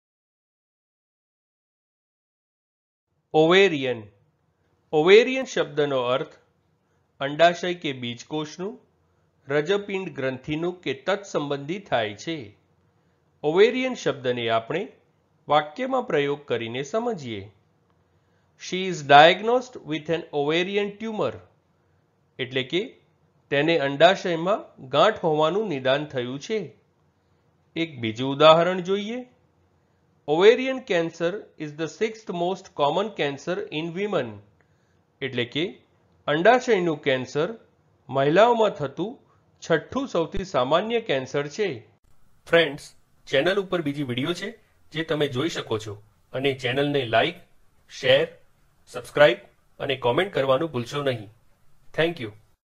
Ovarian Meaning in Gujarati | Ovarian નો અર્થ શું છે | Ovarian in Gujarati Dictionary
આ વિડિયો માં તમે Ovarian નો Gujarati માં અર્થ સમજશો અને એની સાથે Ovarian નું pronunciation પણ શિખશો. એટ્લે કે તમે Ovarian ના meaning ની સાથે સાથે એ પણ શિખશો કે Ovarian ને કેવી રીતે બોલાય, Ovarian ને બોલવાની સાચી રીત કઈ છે.

તો બસ એક મિનટ માં શીખો Ovarian શબ્દ ને.

Ovarian ના આ examples થી સમજીએ

She is diagnosed with an ovarian tumour.

Ovarian cancer is the sixth most common cancer in women.